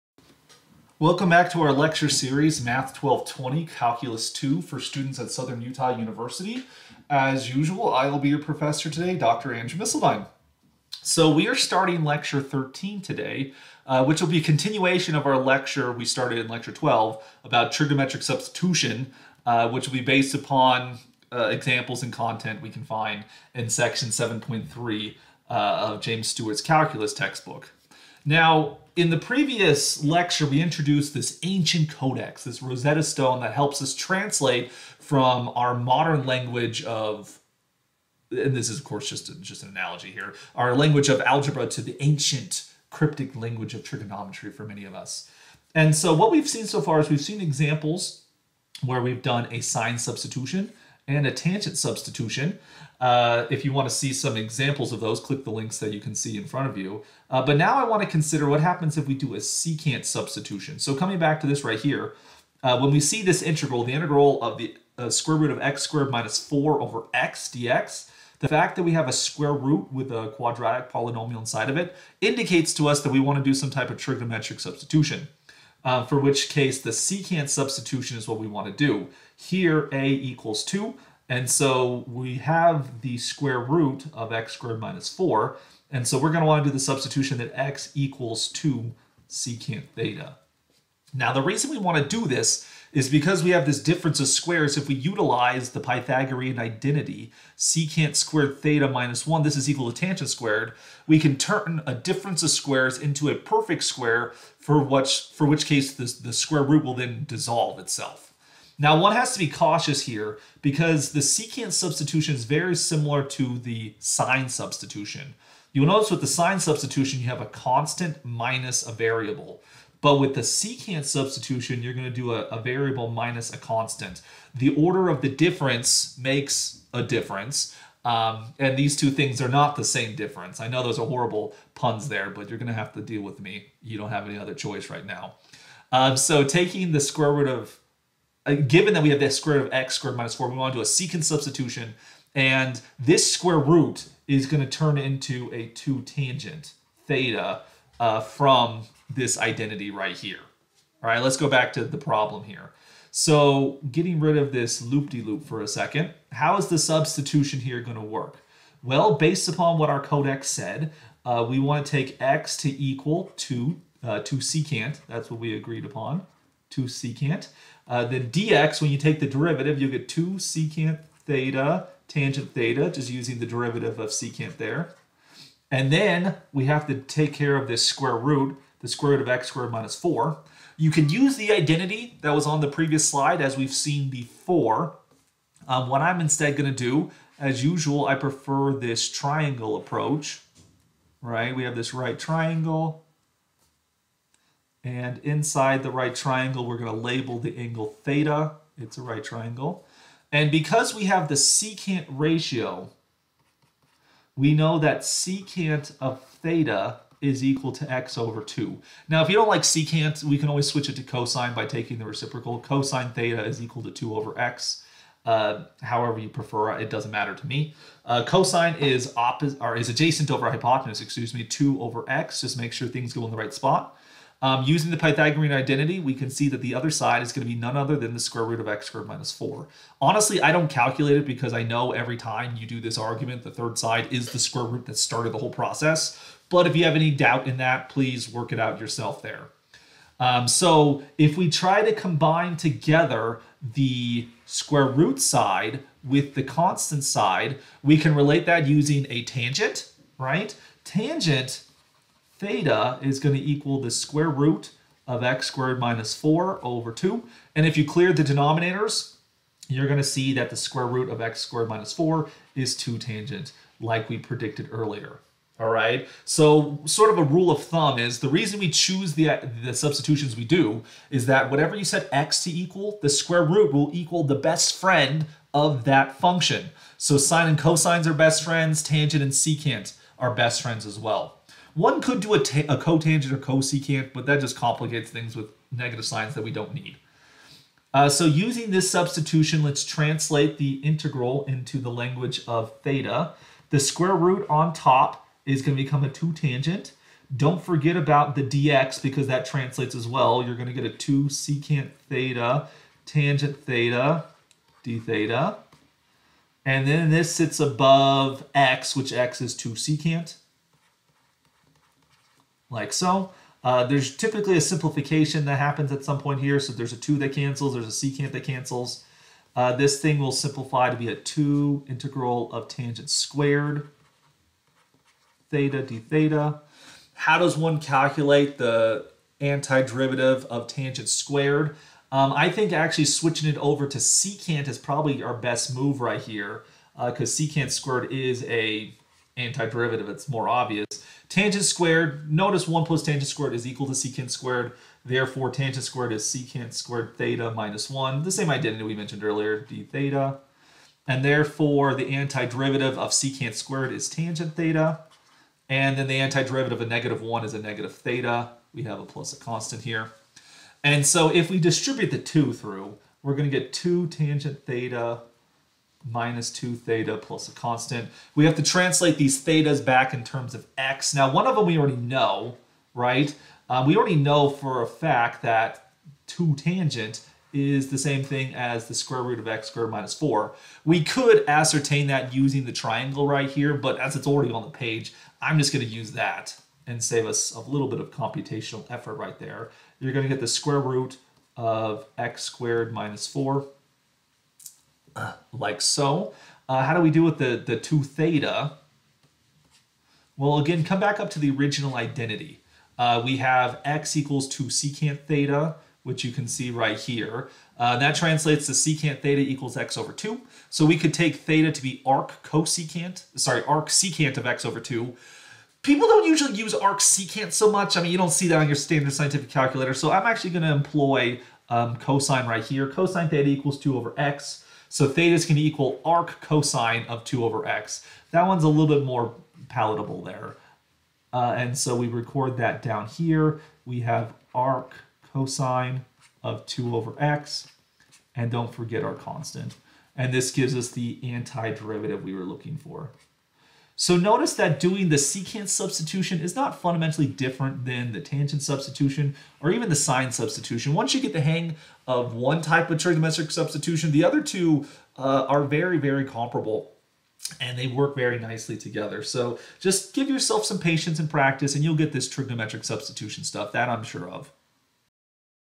Trigonometric Substitution using Secant
In this video, we demonstrate how to use a trigonometric substitution, specifically using a secant substitution, to compute the antiderivative of an algebraic function involving a square root of a difference of squares, the constant coming second.

This is lecture 13 (part 1/4) of the lecture series offered by Dr. Andrew Misseldine for the course Math 1220 - Calculus II at Southern Utah University. A transcript of this lecture can be found at Dr. Misseldine's website or through his Google Drive